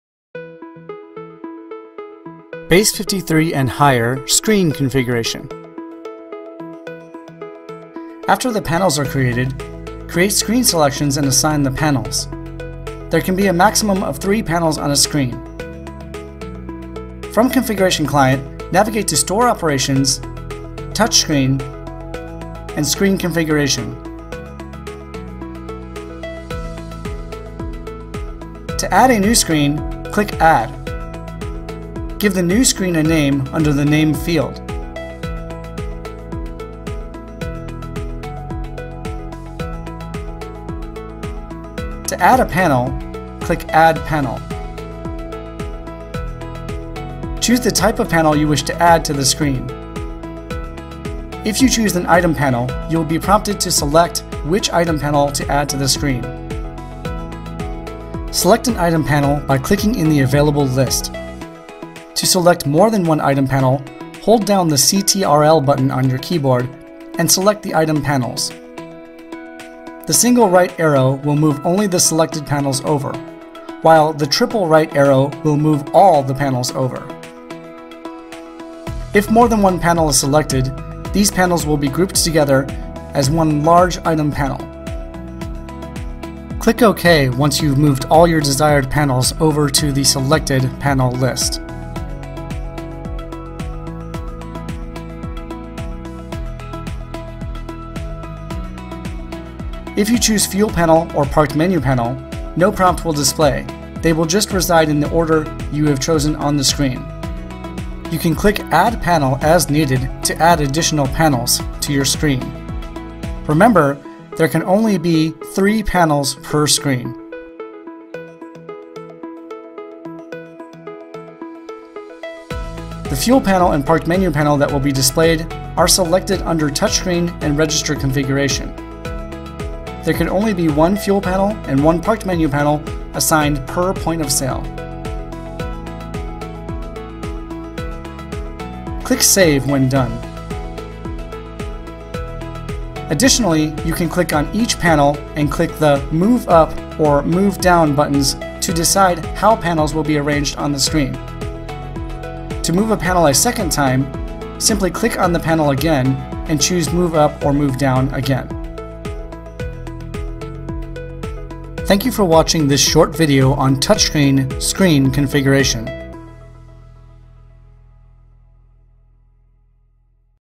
Base 53+ Screen Configuration
This video shows users how to configure Screens for  Points of Sale running software Base 53 and Higher and assign panels to those screens.